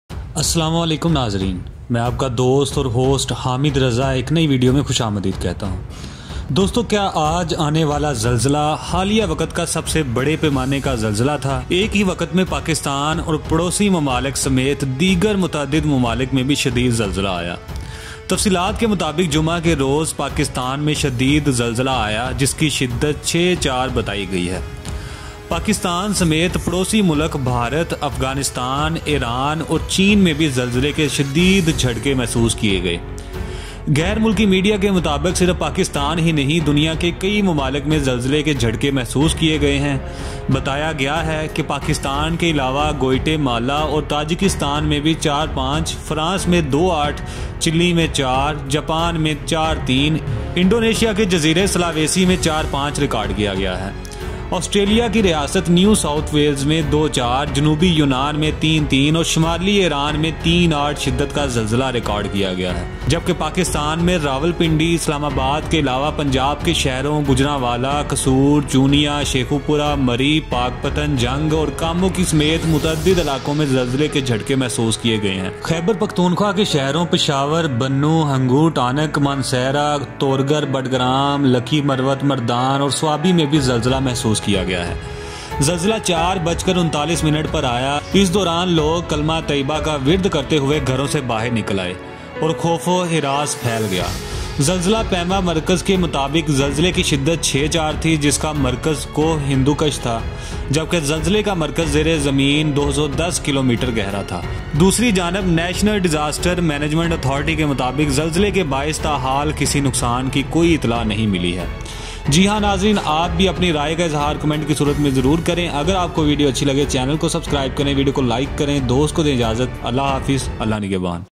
Earthquake in Pakistan today latest news | Earthquake in China, India, Japan, Australia, America
In this video, we discuss the Earthquake in Pakistan's latest news today | Earthquake in China, India, Japan, Australia, America.

Mic: Huawei Mate 10 Lite
Pictures Credit: Google
Software: WonderShare Filmora 9